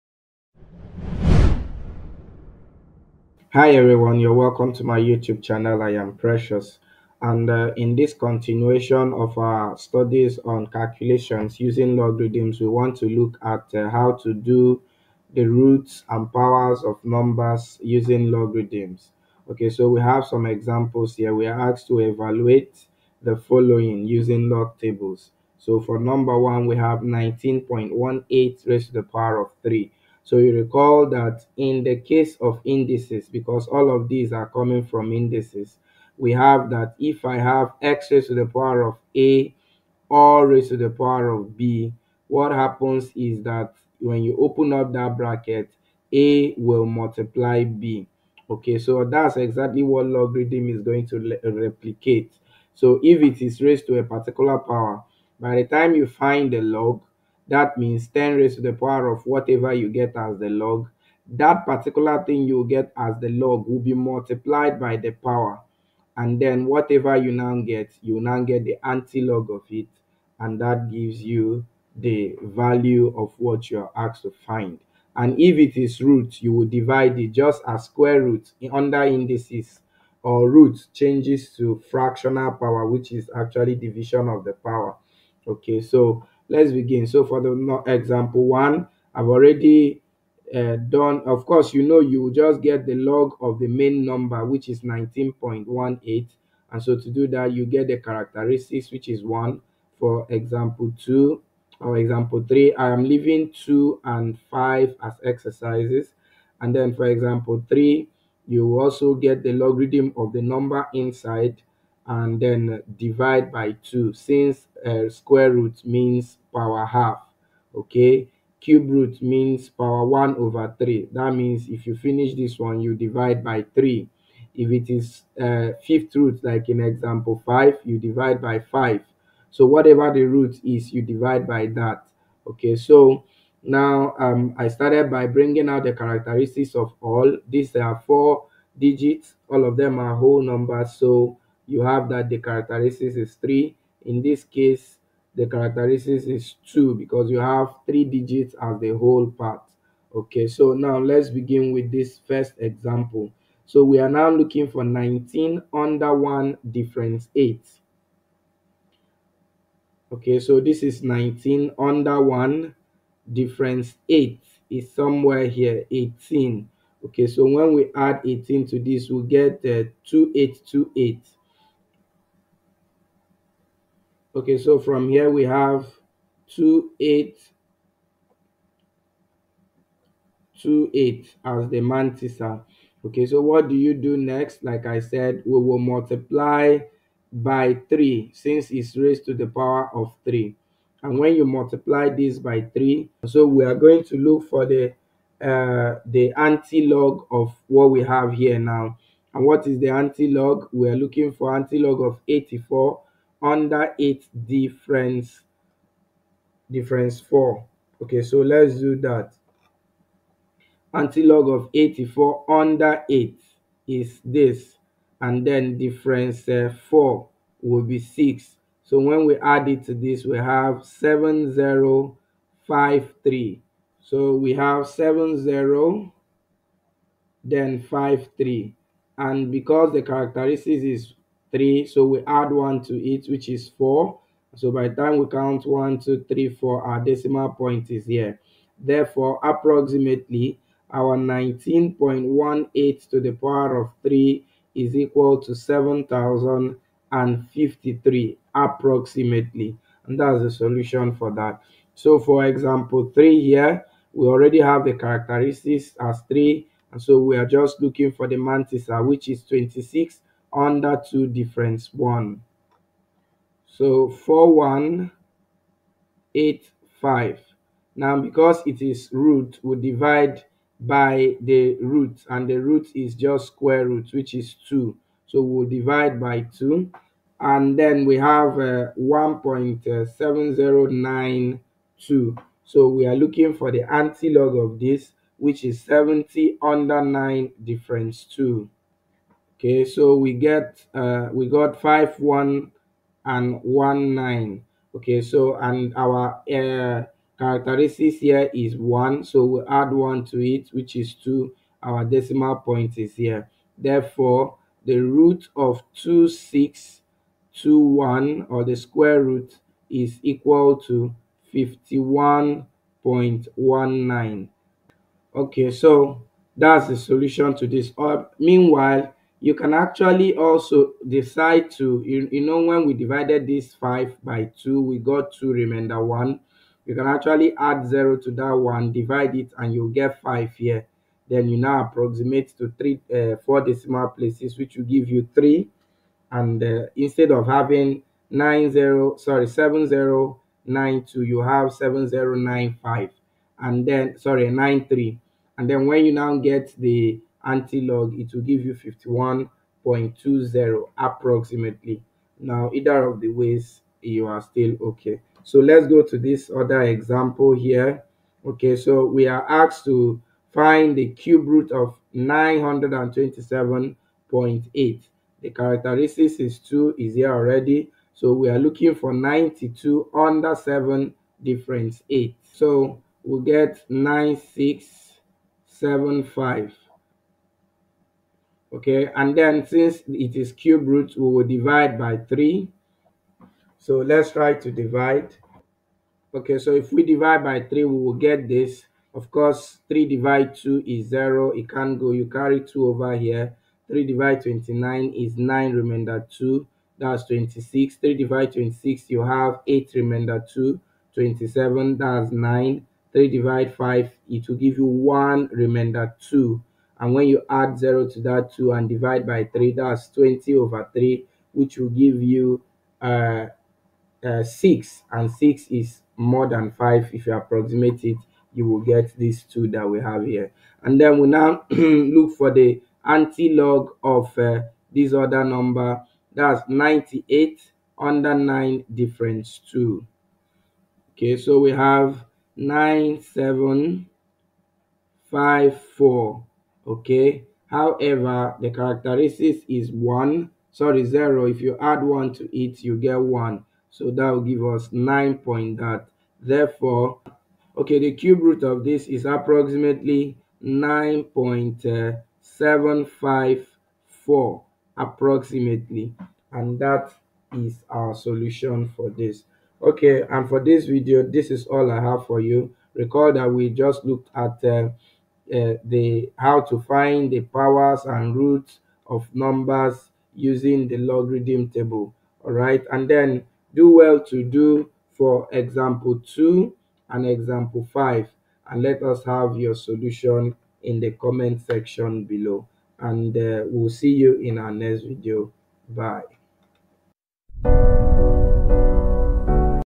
Roots and Powers of Numbers using Logarithms
How to find the powers and roots of numbers using logarithm tables